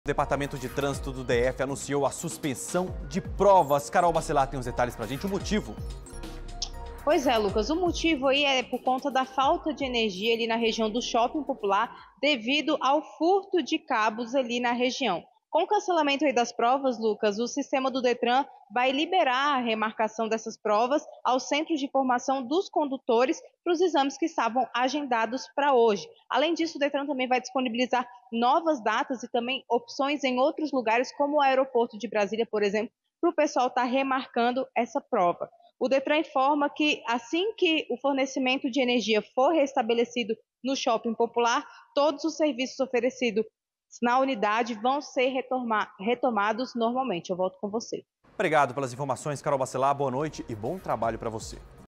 JL - Falta de energia faz Detran-DF suspender provas teóricas
04/11  - Furto de cabos no Shopping Popular causa interrupção de energia.